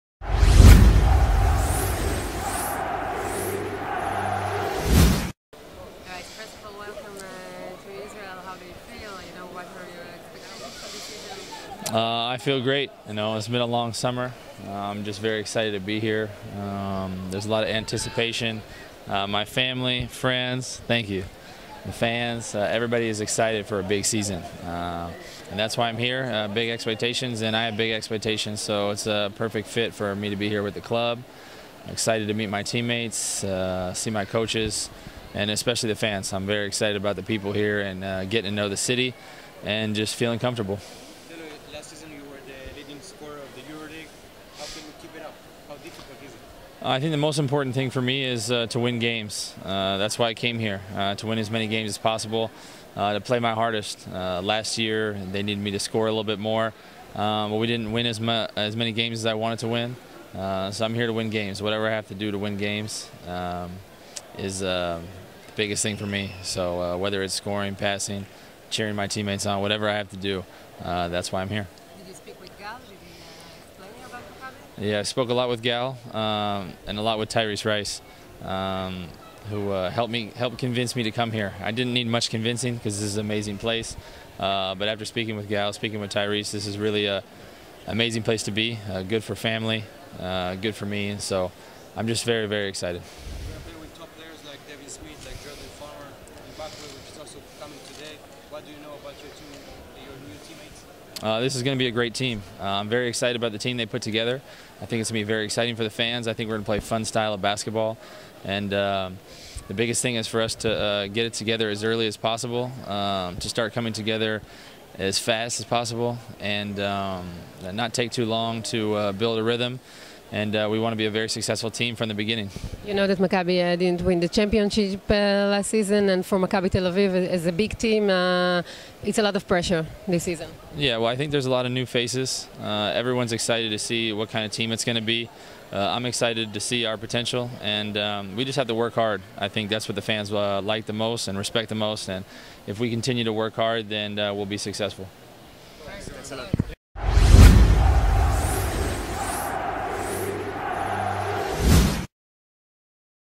Taylor Rochestie lands in Tel Aviv
מלך הסלים של היורוליג, טיילור רוצ'סטי, נחת הבוקר (א') בישראל לקראת פתיחת האימונים בהמשך השבוע: "אני מרגיש נהדר, יש לי הרבה ציפיות במכבי ולמכבי יש ציפיות כך שזאת התאמה מושלמת".